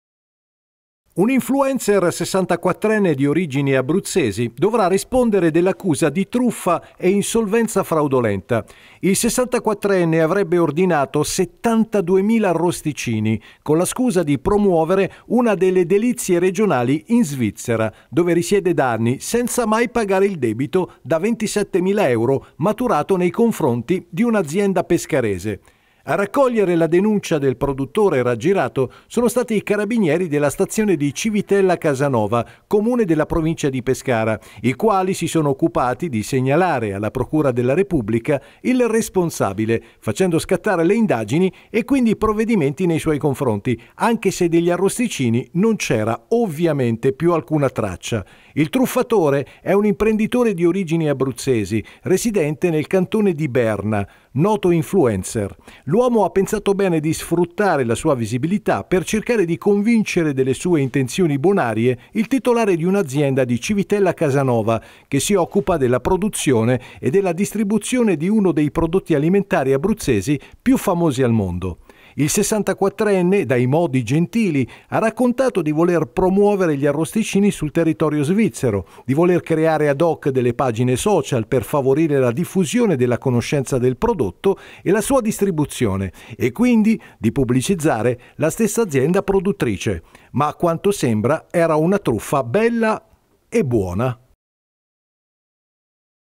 Pescara. Truffa degli arrosticini: influencer denunciato
Avrebbe ordinato 72mila arrosticini con la scusa di promuoverli in Svizzera, dove risiede da anni, senza mai pagare il debito maturato nei confronti di un'azienda abruzzese. Un influencer è stato denunciato per truffa.